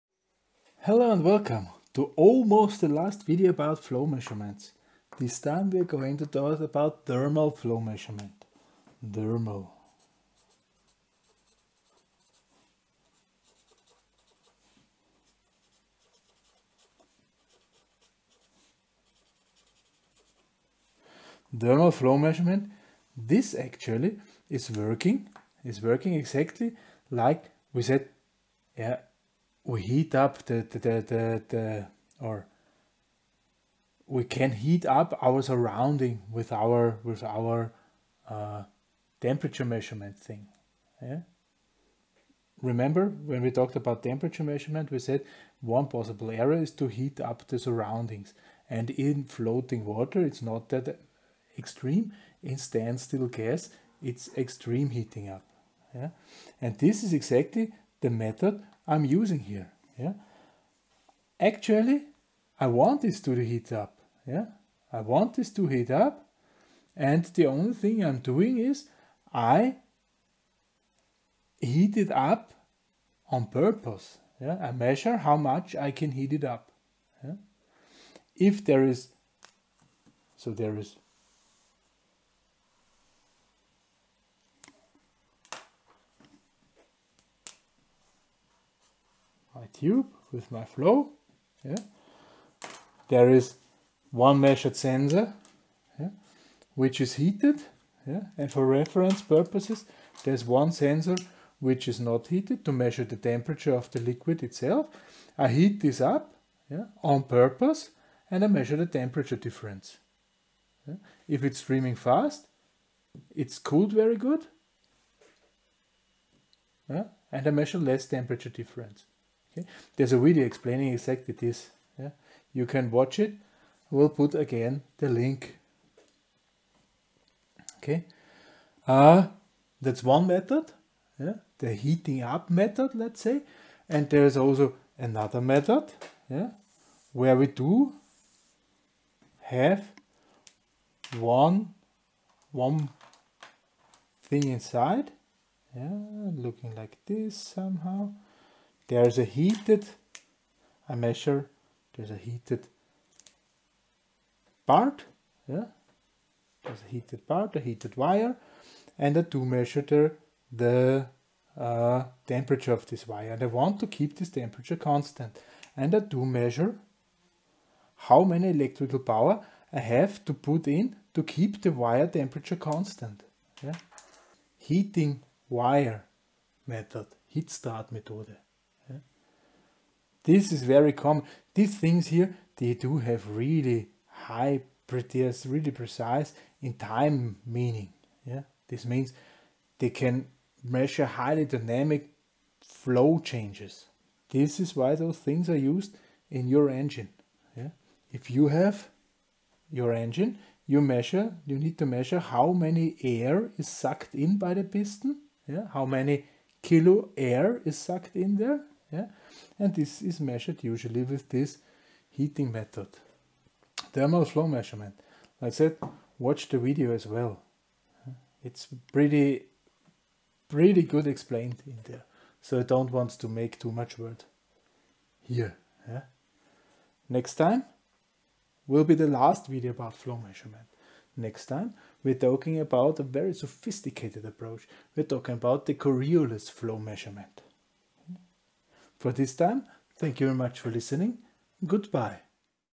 Measure 56: Thermal Flow Measurement (Old)
This video explains the thermal mehtode for flow measurement